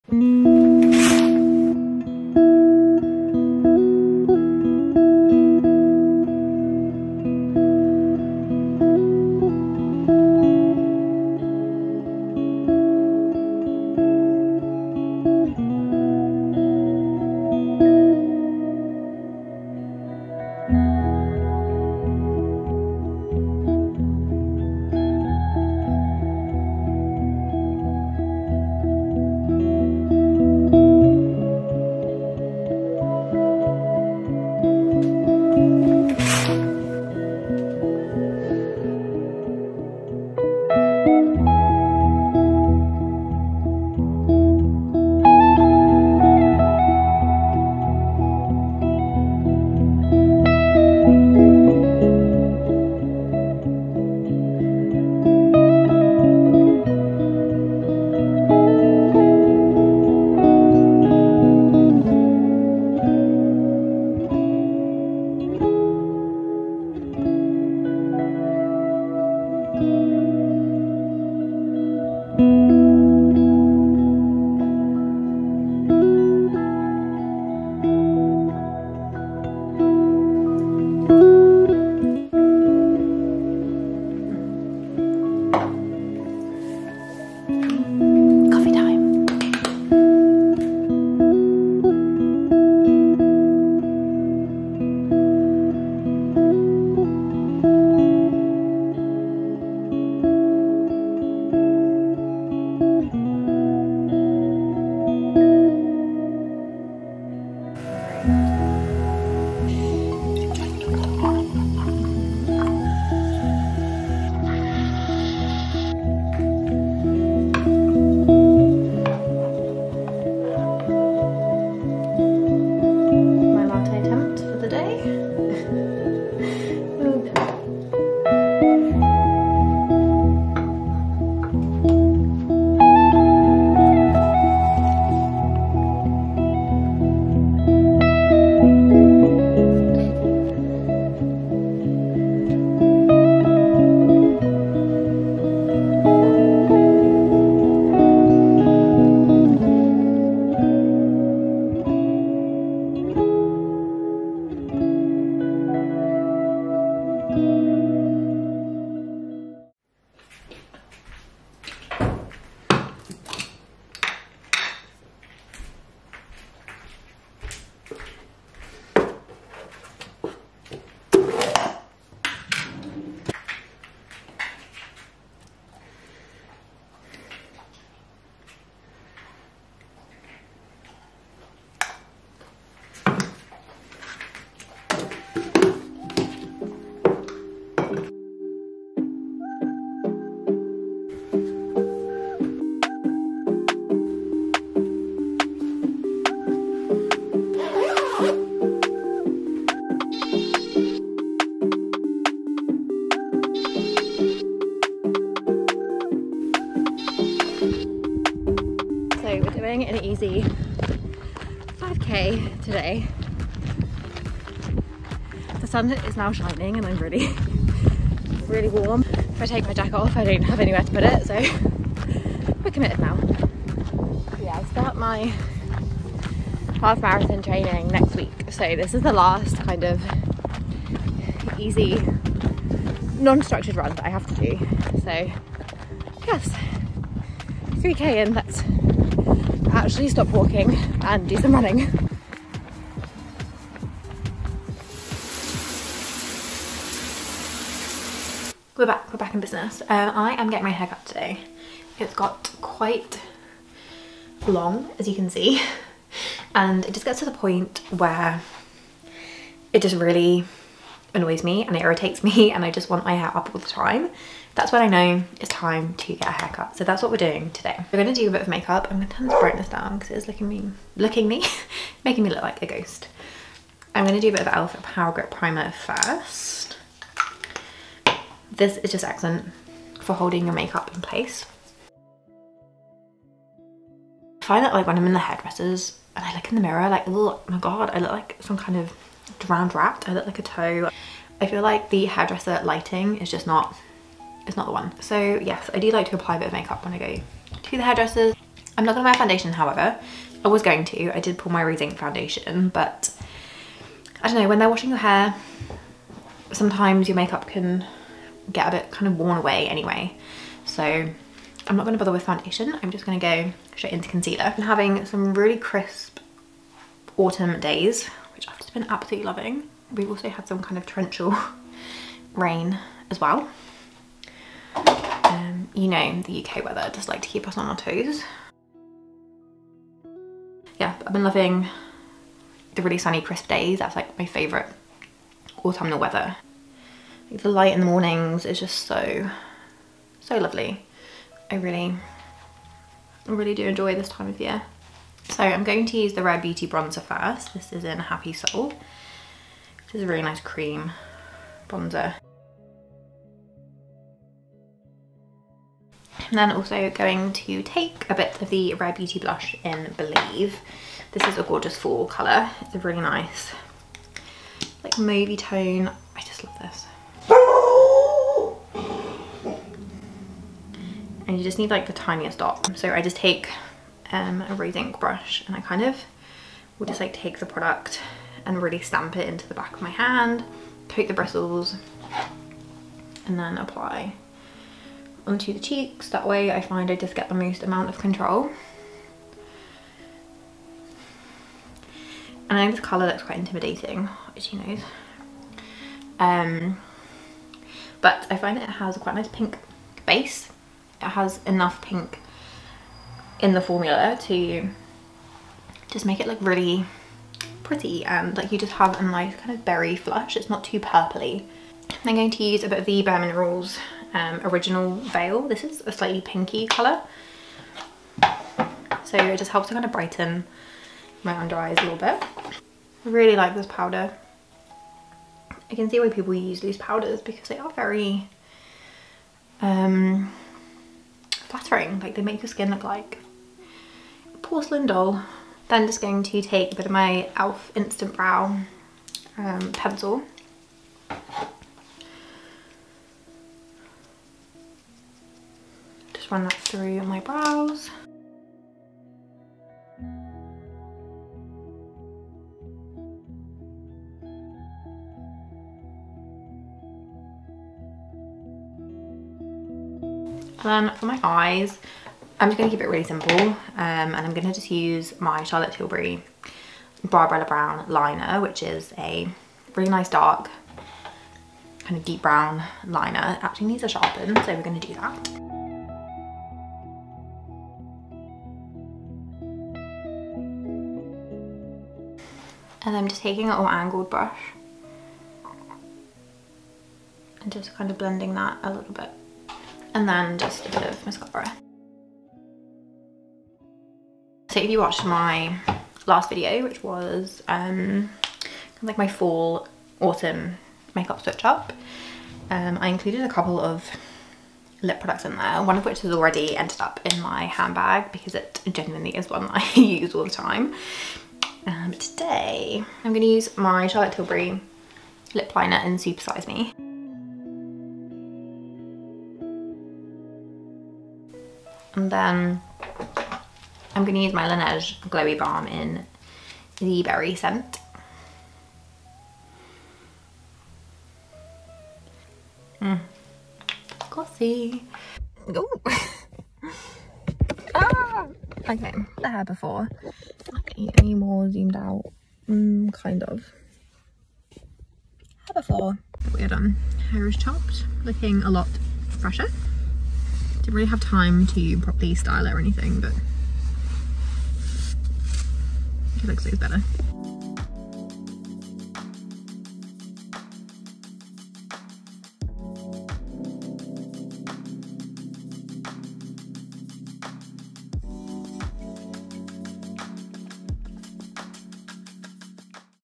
AUTUMN/FALL MORNING ROUTINE | *Cosy* Morning & Half Marathon Training
*Beauty of Joeson Ginseng Essence
elf Power Grip Primer
NYX Bare With Me Concealer
Bare Minerals Original Veil
Rare Beauty Bronzer Stick - Happy Soul
Rare Beauty Liquid Bronzer - Believe
elf Instant Lift Brow Pencil
Charlotte Tilbury Kohl Liner - Barbarella Brown
Maybelline Sky High Mascara
Charlotte Tilbury Lip Liner - Supersize Me
Laneige Glowy Balm - Berry

Other details:
Filming Camera - Canon M50